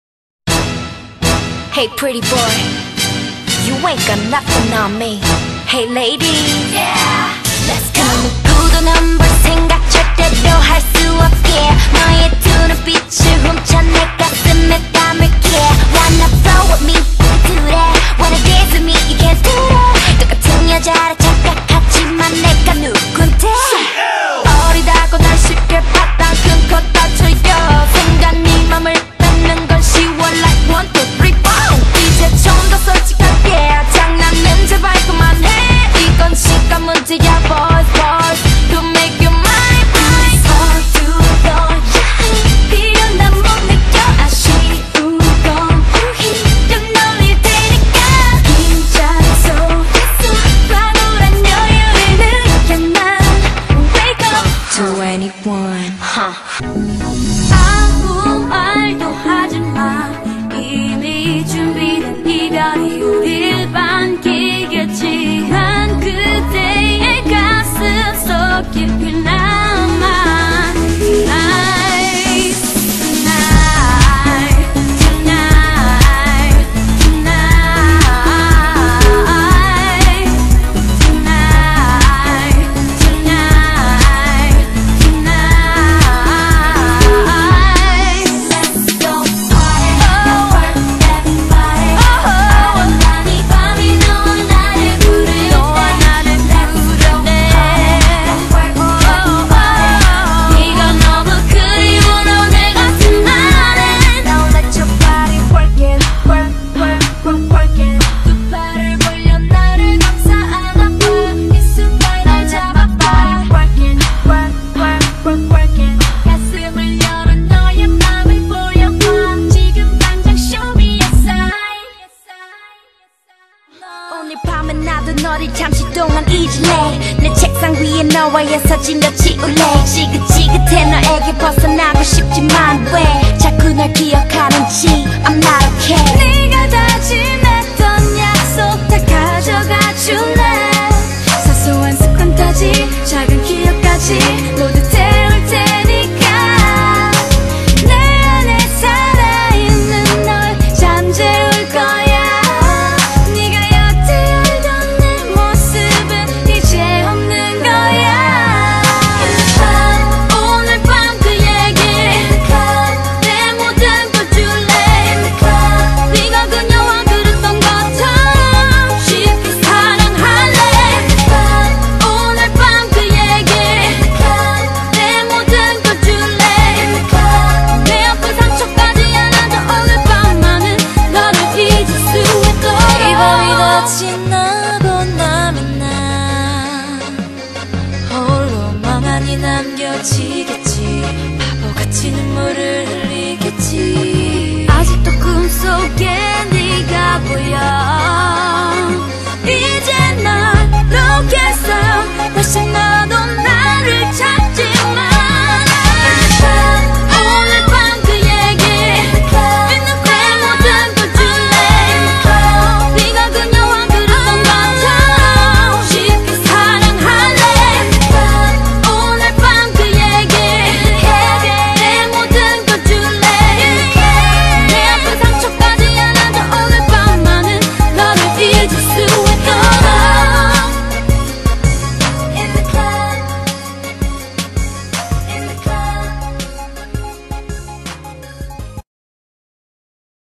2ne1 medley
it is a song video for my friend Niki
I hope she likes it :)